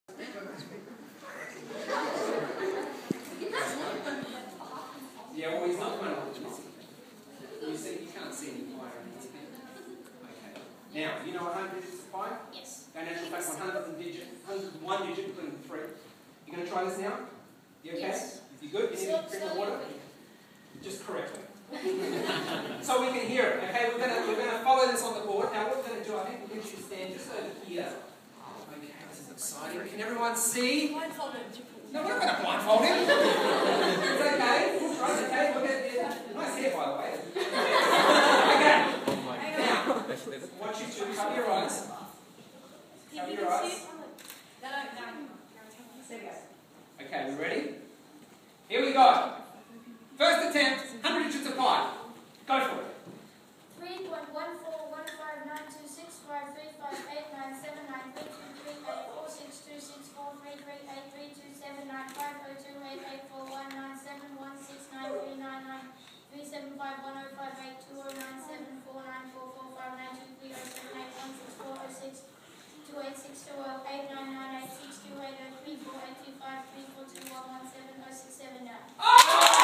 100 digits of pi...easy
Pi Day celebrations at the Australian Museum.